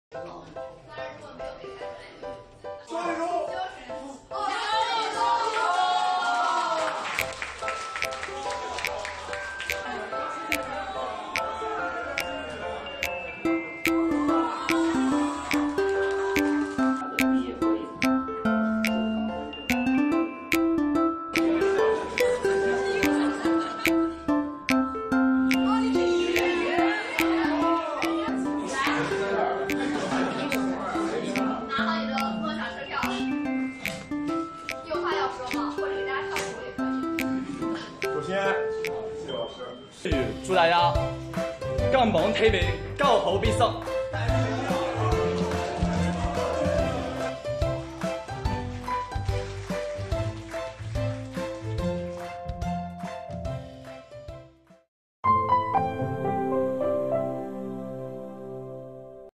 山东高三学子的最后一课：晒晒“成长盲盒”
6月4日，山东省实验中学高三学子迎来高考前“最后一课”，老师和家长们策划了一场“成长盲盒”活动，在课堂上晒出学生们的童年照，让他们一起猜猜“他是谁”，借此机会缓解考生紧张的备考情绪。老师还在现场赠送学生“梦想车票”、毕业纪念扑克和印有祝福语的“高考必胜面”。